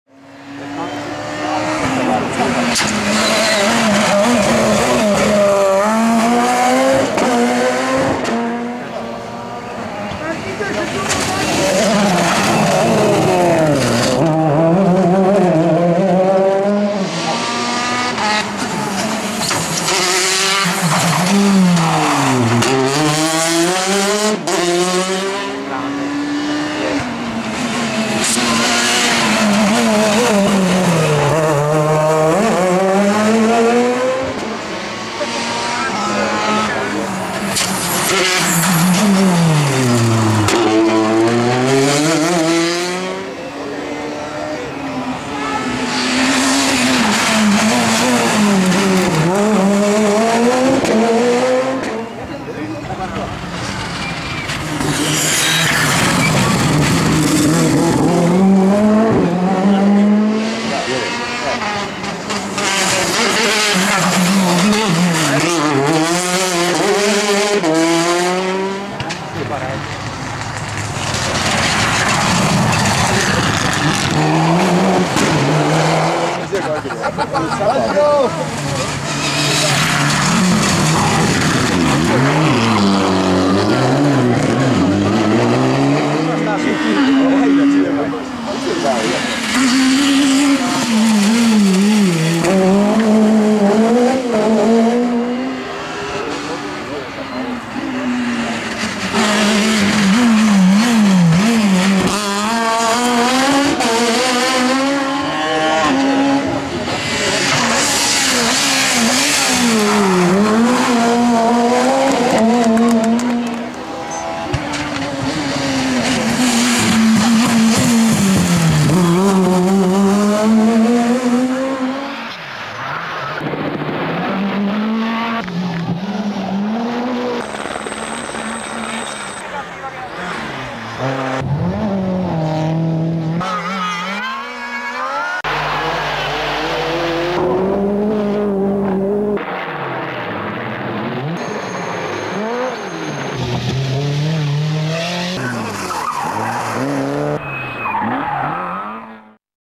30° Rally Bellunese 2015 PS1 Lentiai bivio VIDEO SI
Cari appassionati, ecco la terza telecamera sulla ps 1 Lentiai, filmato il bivio, al 30° Rally Bellunese 2015, primi 15 concorrenti, buon divertimento!!!